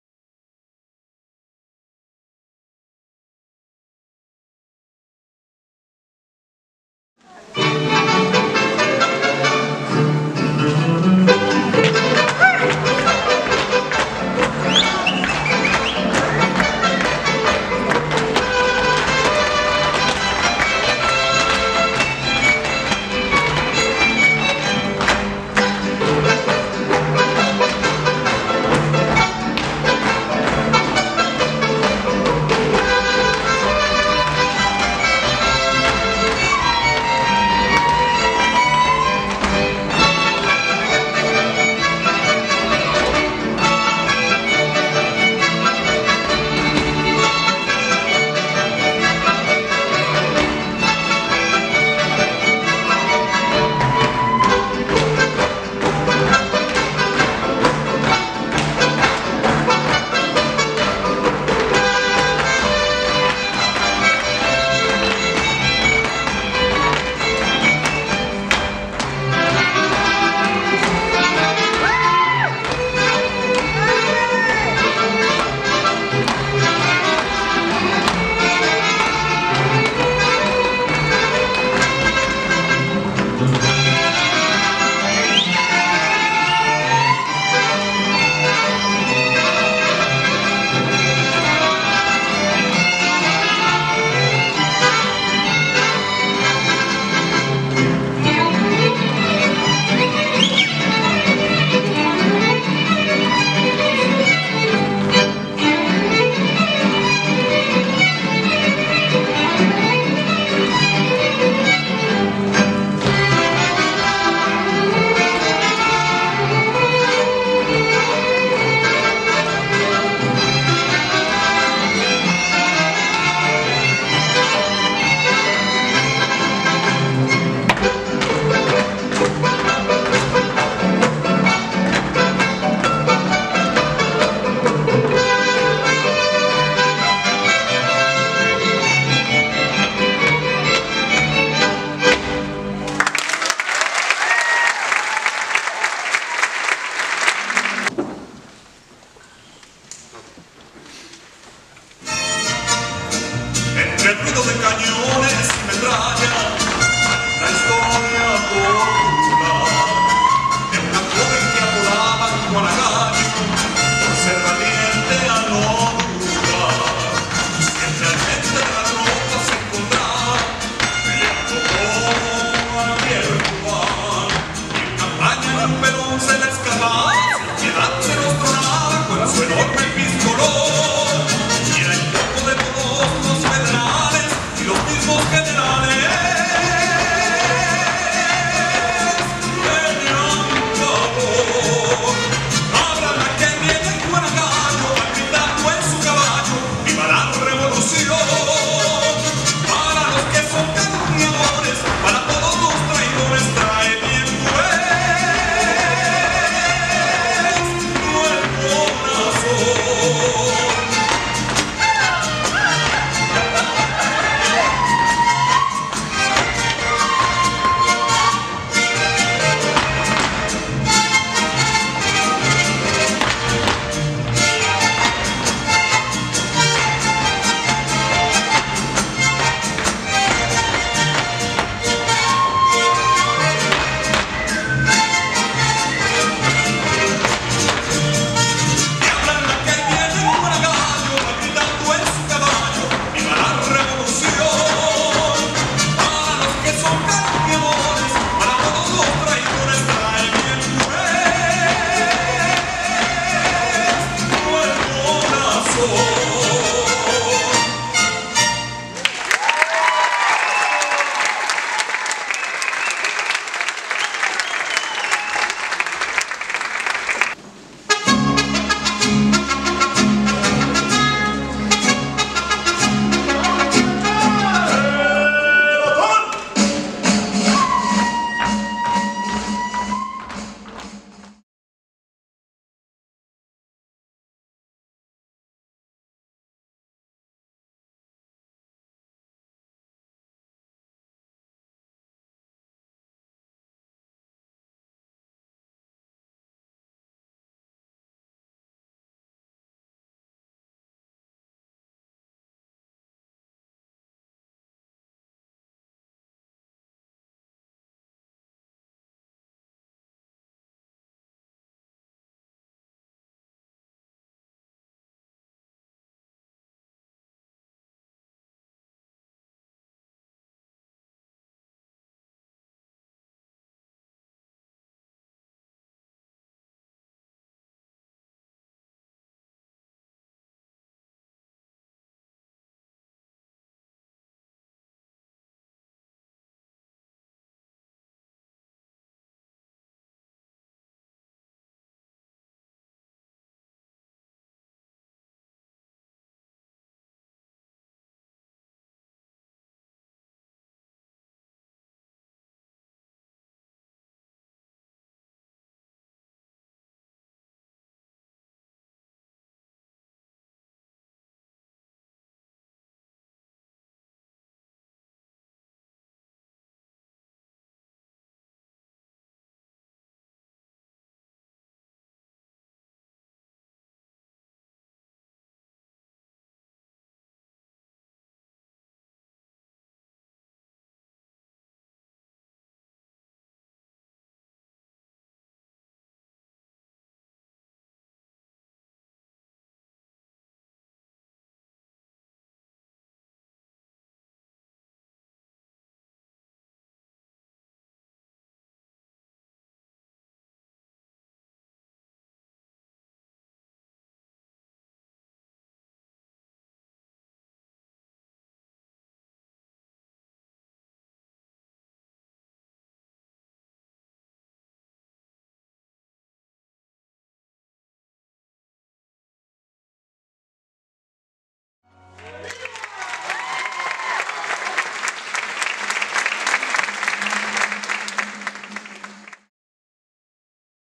Revolucion - Ballet Folklorico de UCI 2019 Spring Recital
Ballet Folklorico de UCI performs Revolucion at their 39th Annual Spring Recital (2019)